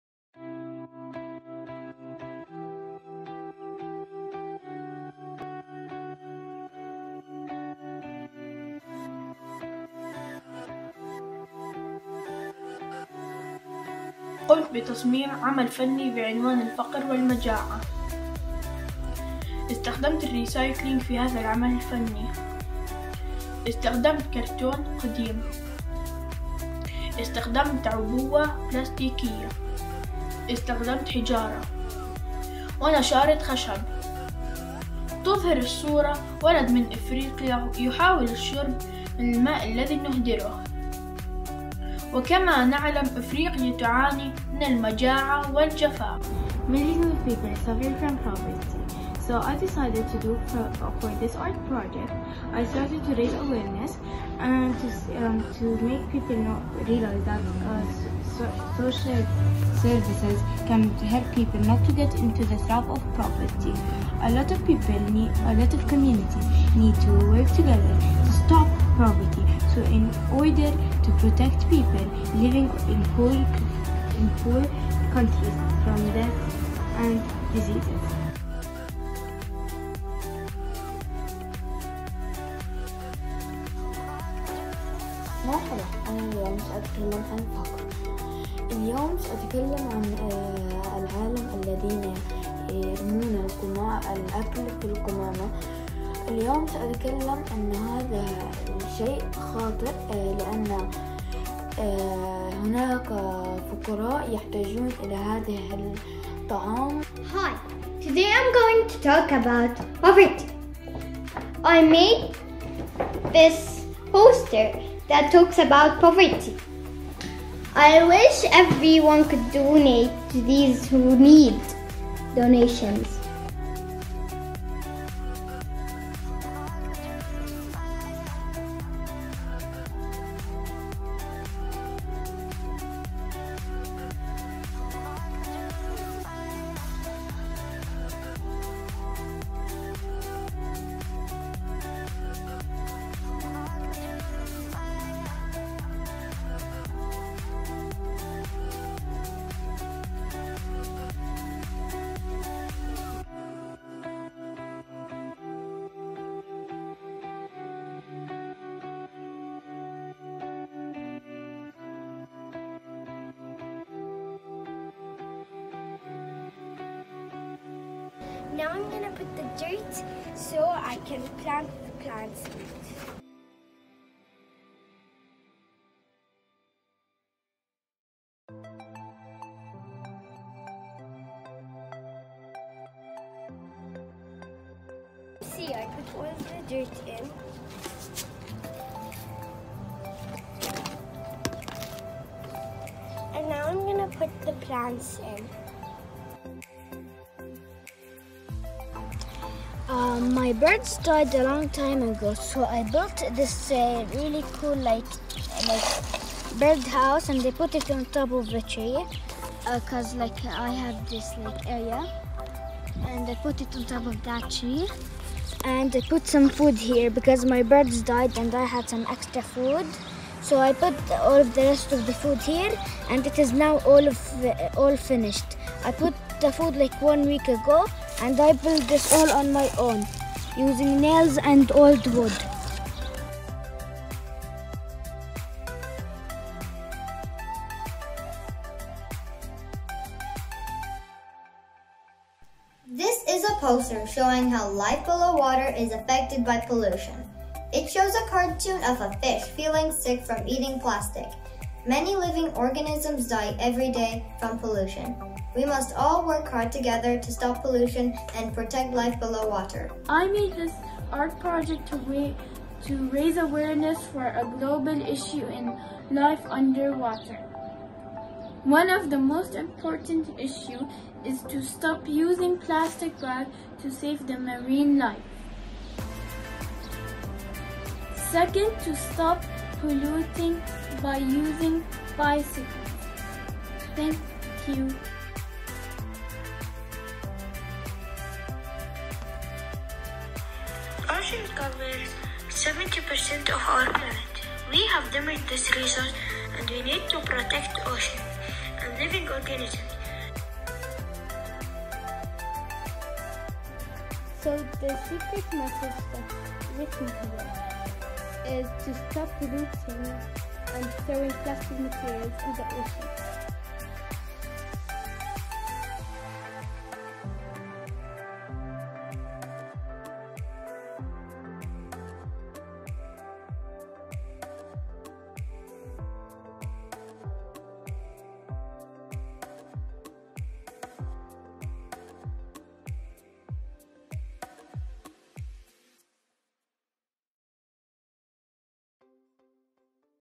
Grade 5 Art - PYP Exhibition
Art as a language!

Our fifth graders showcase their creative artworks during their pyp exhibition, expressing their understanding and feelings about different life issues.